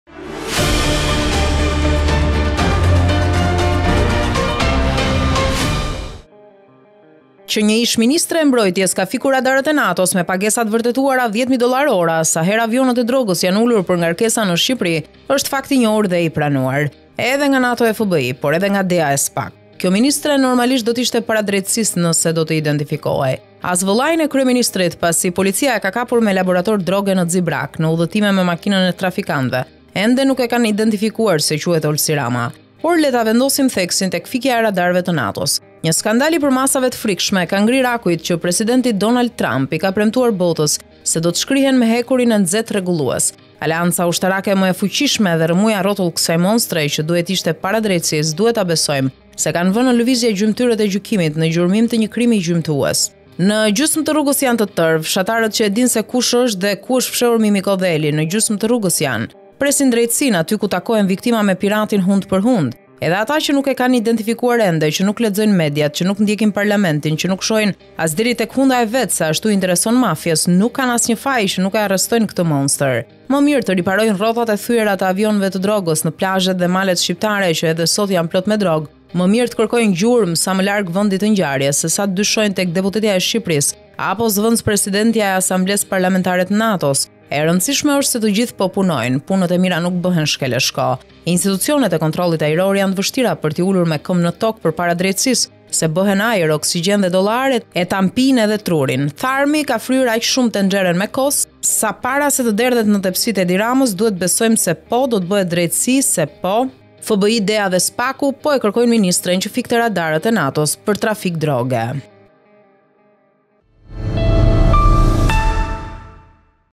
FBI, DEA dhe SPAK, po kërkojnë ministren që ka fikur Radarët e NATO-s për trafik droge!
Që një ish ministre e mbrojtjes ka fikur radarët e NATO-s me pagesa të vërtetuara 10 000 dollarë ora, sa herë avionët e drogës janë ulur për ngarkesa në Shqipëri, është fakt i njohur dhe i pranuar, edhe nga NATO e FBI, por edhe nga DEA e SPAK.

Kjo ministre, normalisht do të ishte para drejtësisë, nëse do të identifikohej.

As vëllain e kryeministrit, pasi policia e ka kapur me laborator droge në Xibrakë, në udhëtime me makinën e trafikantëve, ende nuk e kanë identifikuar se quhet Olsi Rama.